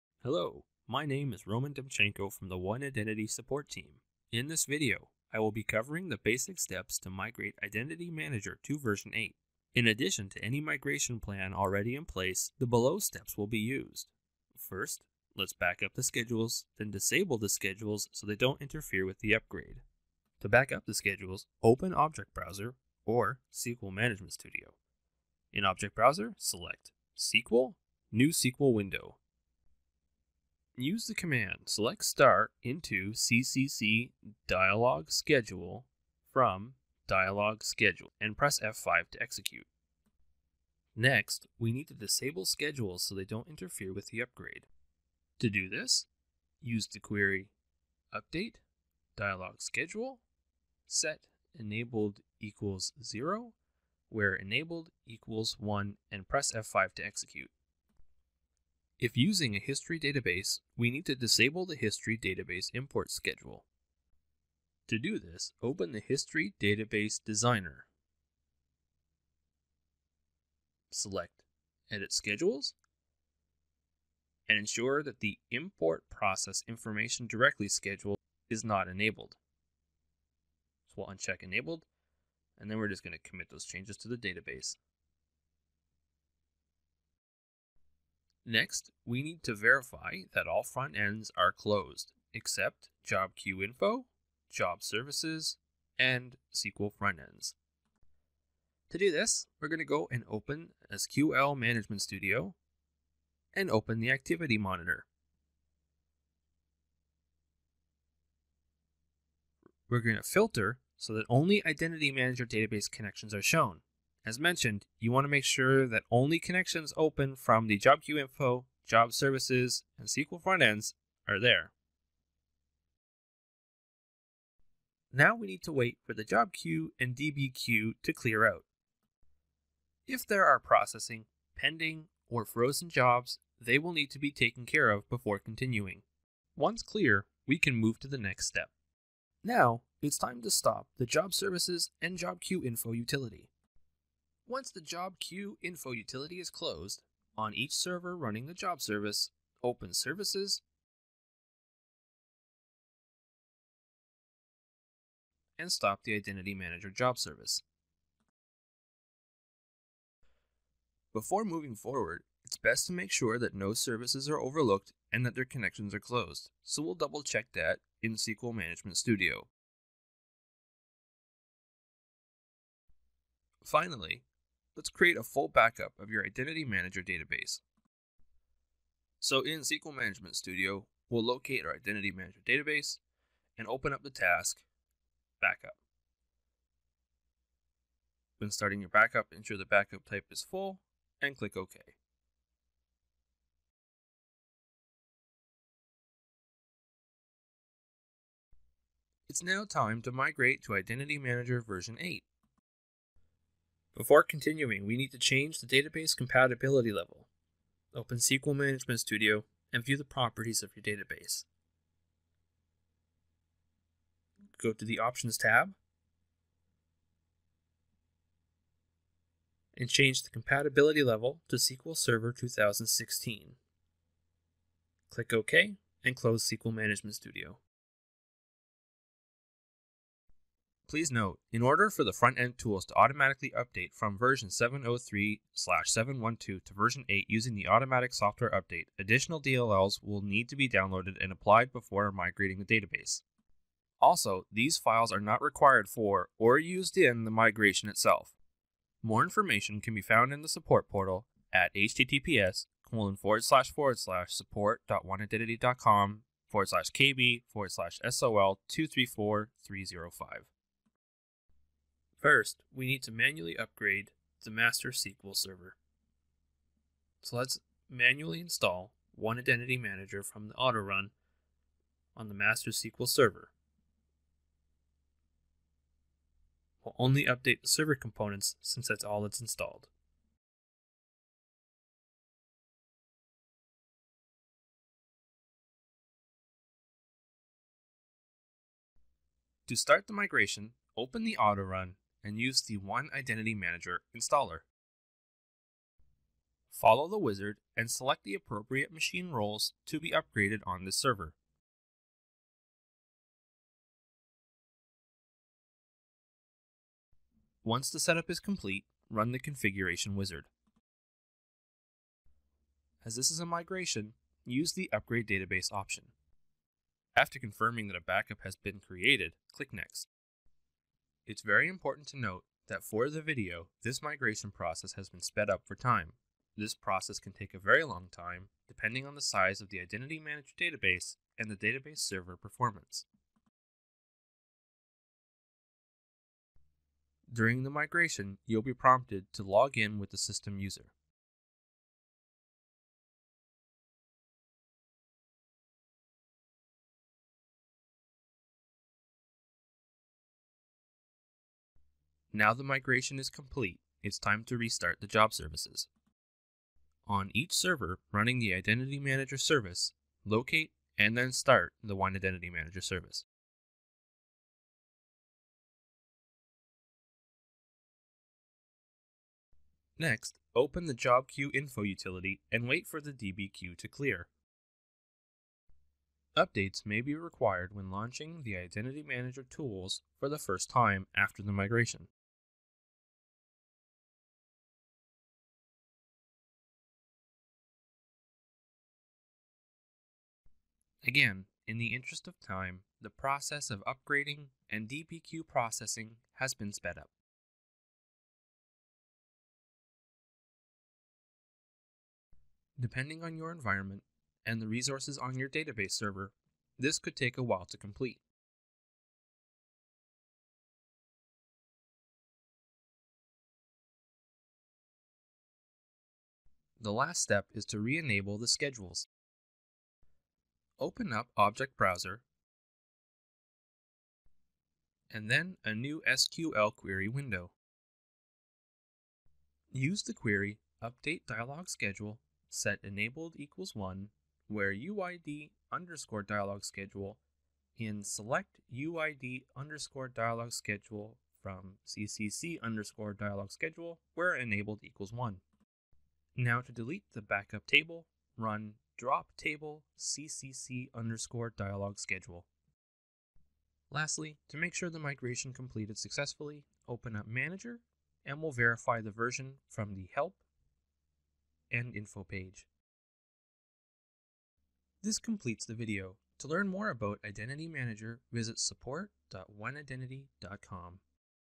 Upgrading to Identity Manager 8 from versions 7.0.3/7.1.2
To help ensure a smooth migration of Identity Manager, what are some steps that should be followed when upgrading to version 8 from versions 7.0.3/7.1.2?